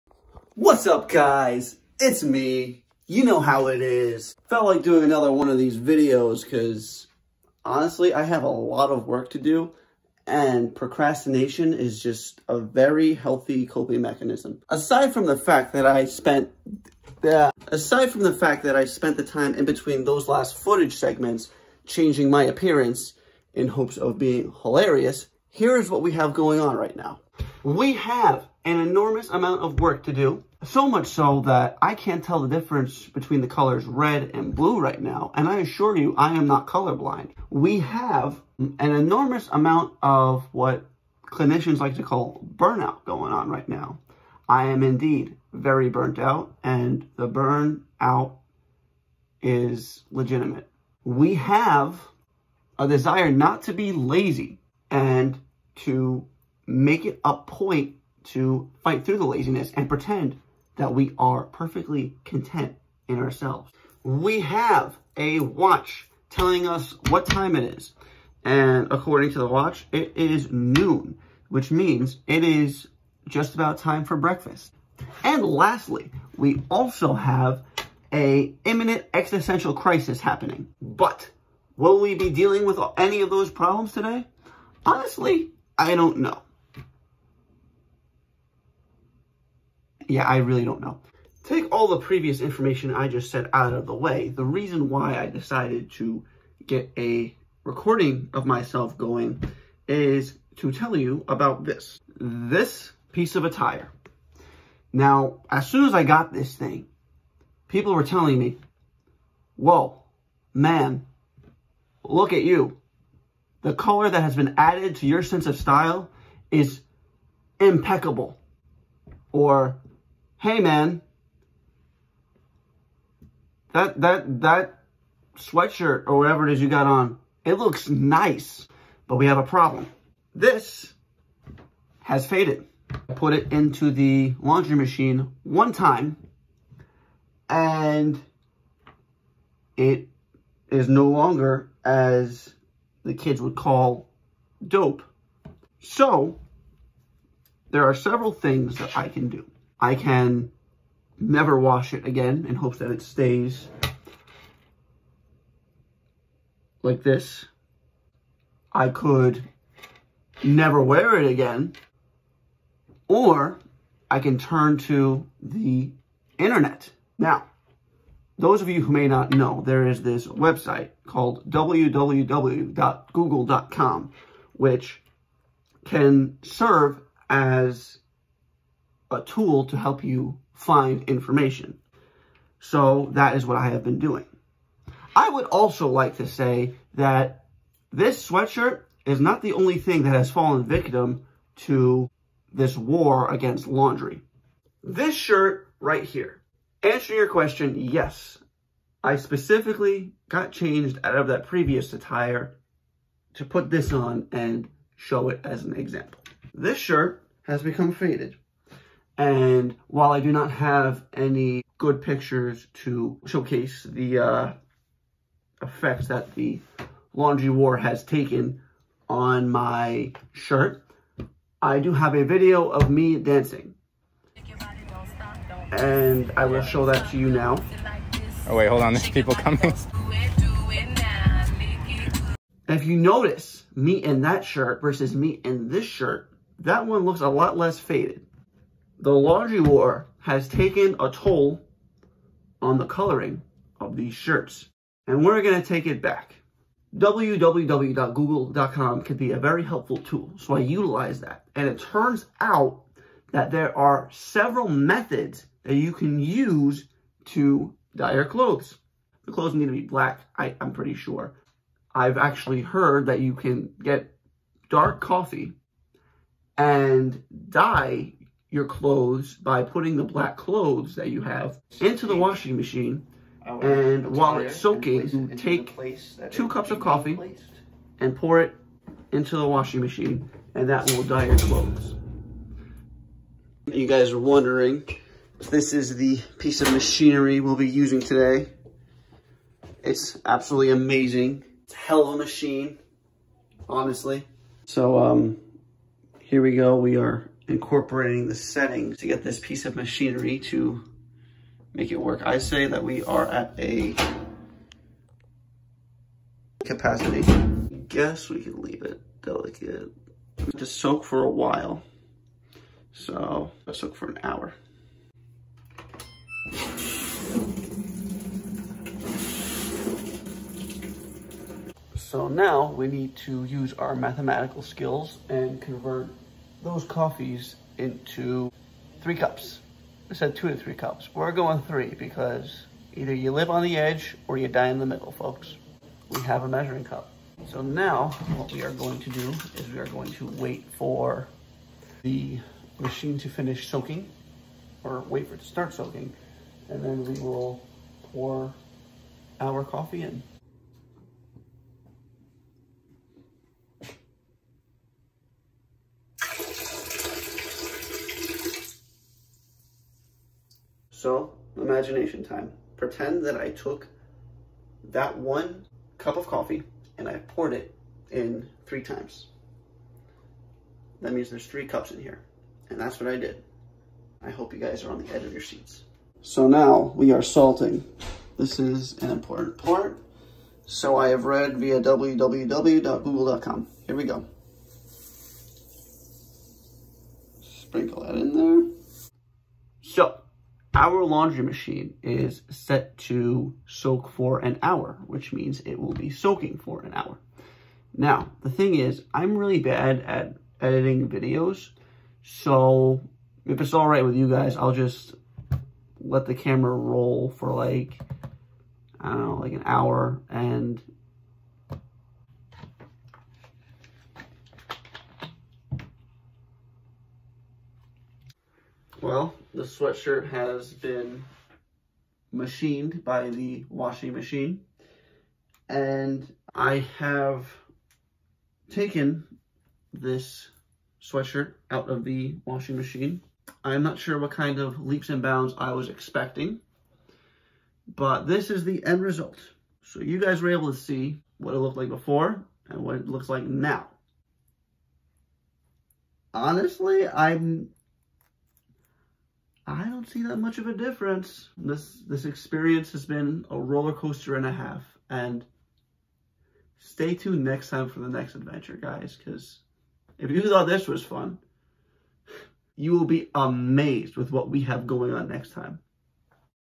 THIS Is What Happens When You Put Coffee In The Washing Machine To Dye Your Clothes!!!
Have you wanted to put coffee in the dishwasher?Have you ever wondered if using coffee to dye your clothes would work? I hope this footage quenches your thirst for knowledge...